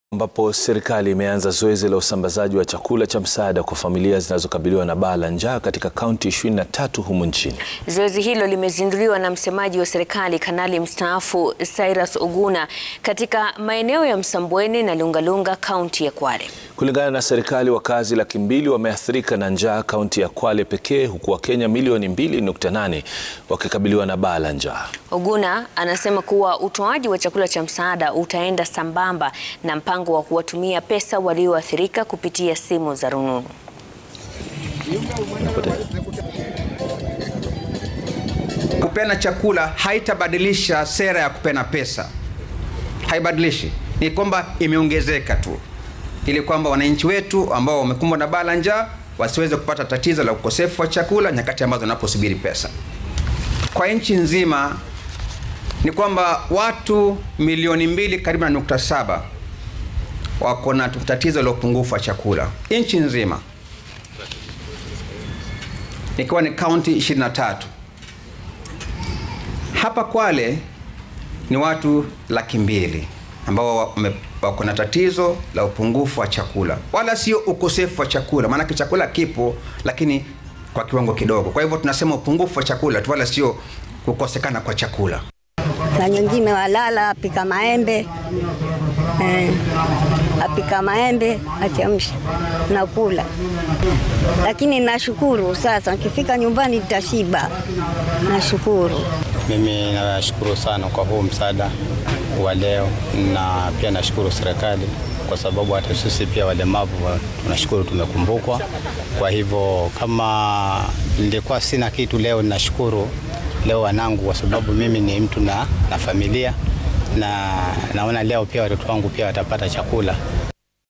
Serikali imeanza zoezi la usambazaji wa chakula cha msaada Kwale
Familia milioni mbili zimeathirika na njaa  kaunti 23
Wakaazi laki mbili wameathirika na njaa kaunti ya Kwale pekee